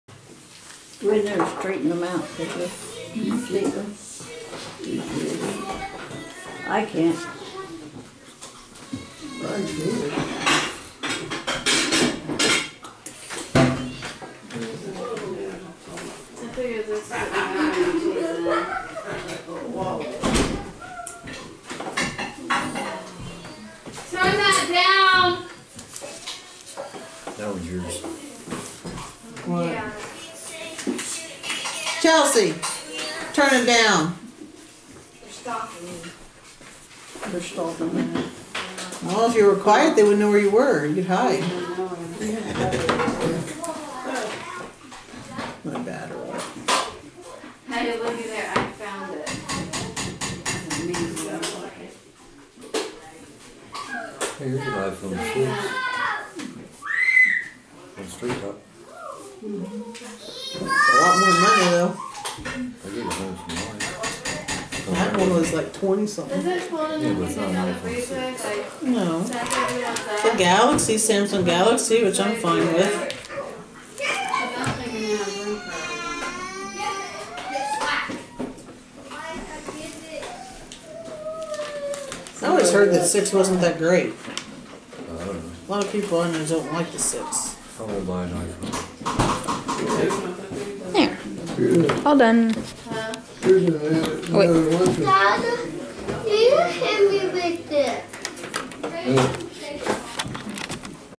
Me Drawing
I just drew a simple picture at I had in my head. Its just a girl.